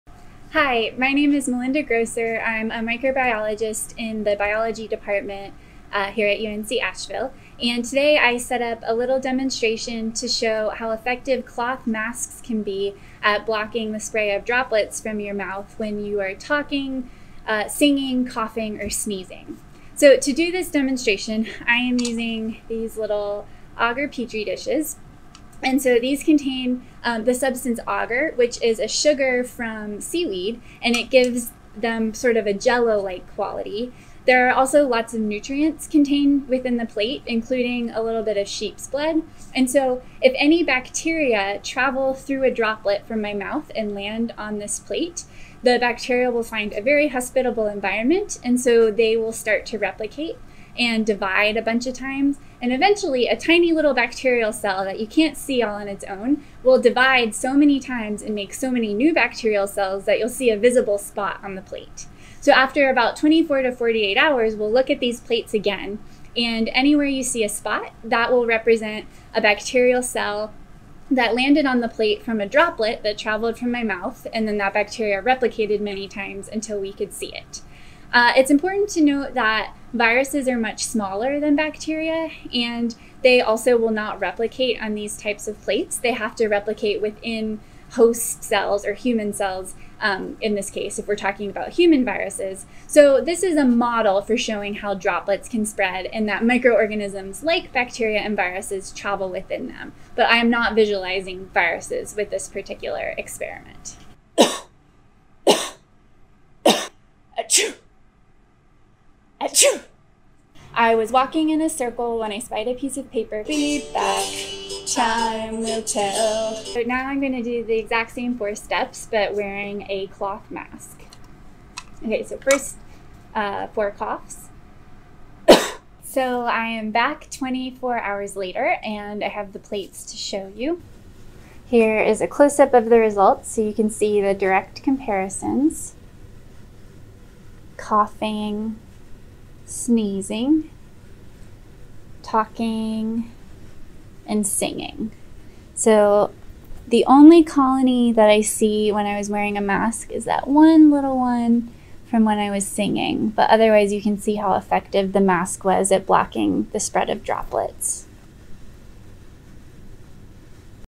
Melinda Grosser Mask Test
UNC Asheville Biology professor Melinda Grosser conducts a test to show the efficacy of wearing masks to prevent the spread of Covid-19.